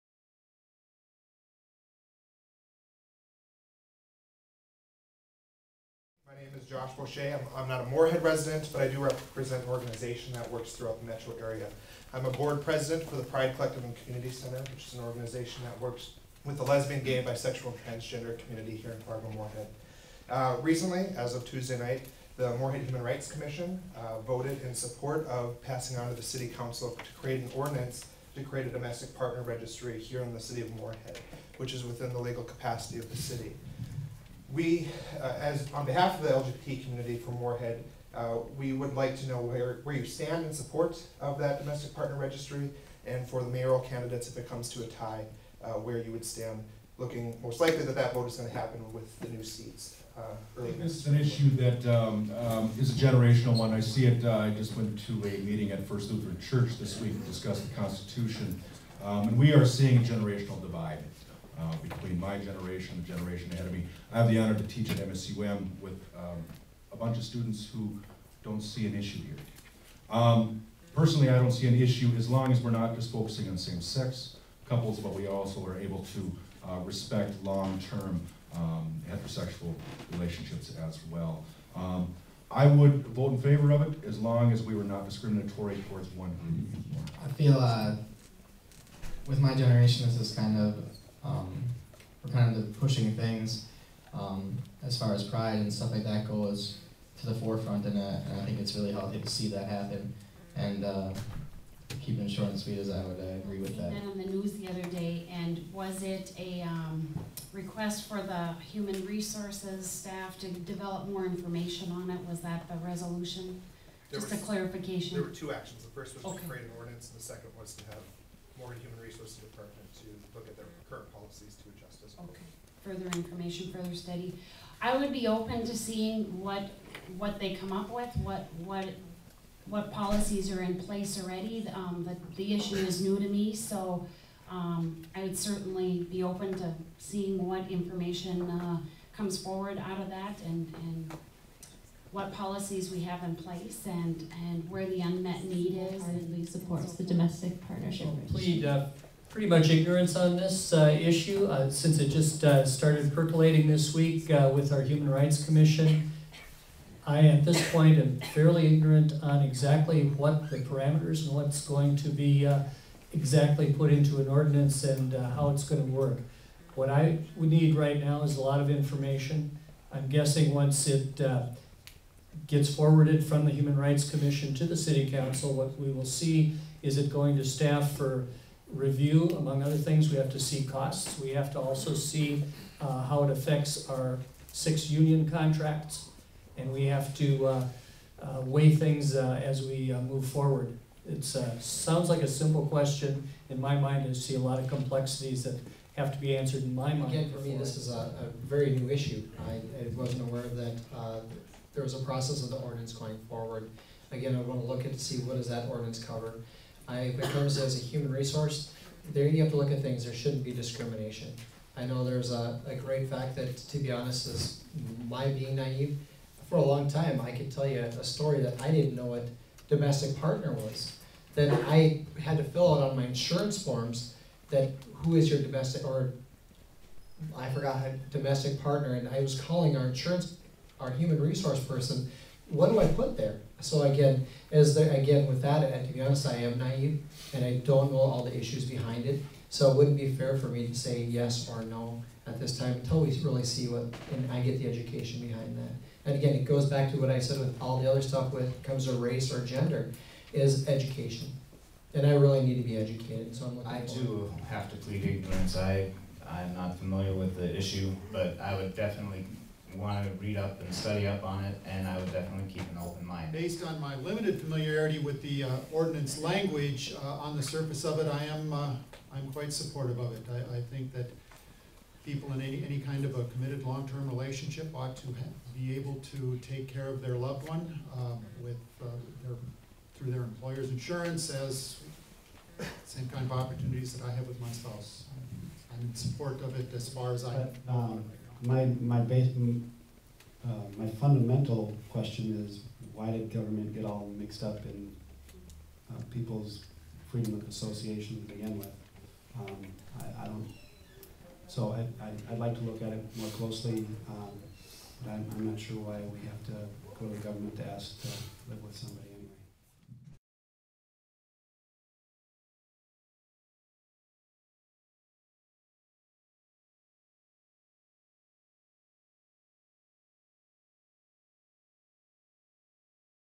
Candidates address Domestic Partnership Registry Question
Moorhead Mayoral and City Council Candidates address  Domestic Partnership Registry Question